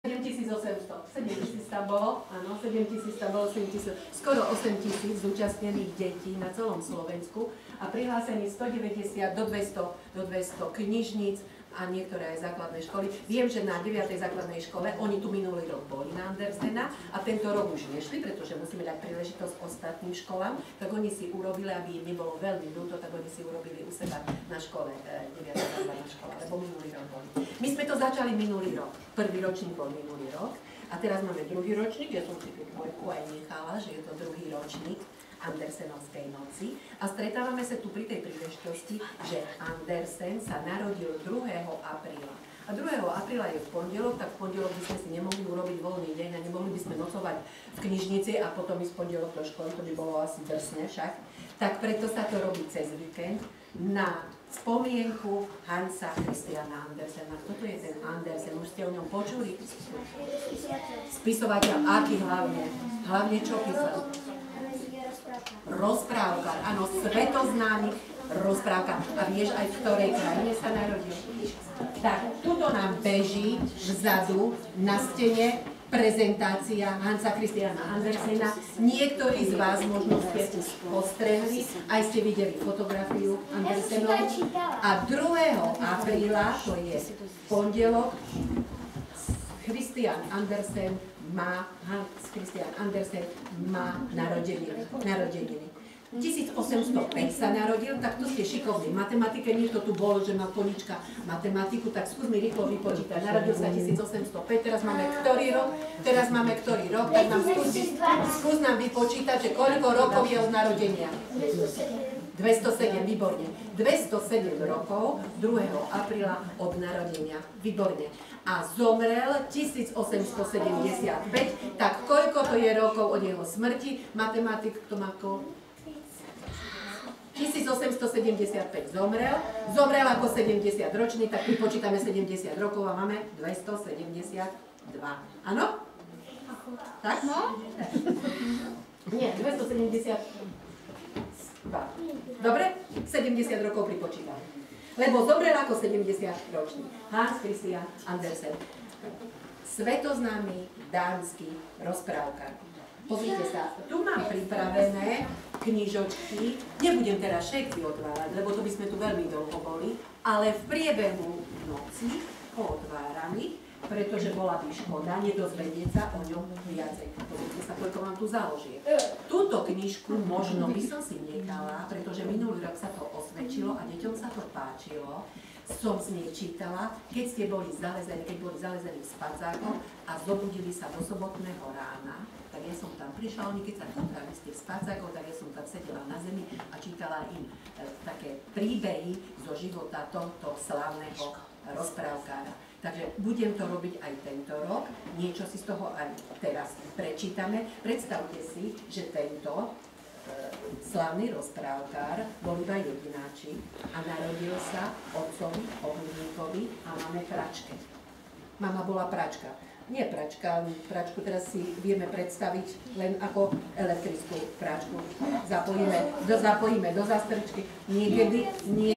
Noc s Andersenom 2012 (reupload)
Krajská knižnica Ľ. Štúra vo Zvolene otvorila bránu do rozprávkovej noci z piatka 30. na sobotu 31. marca 2012 veselej chase tridsiatich zvolenských žiačikov.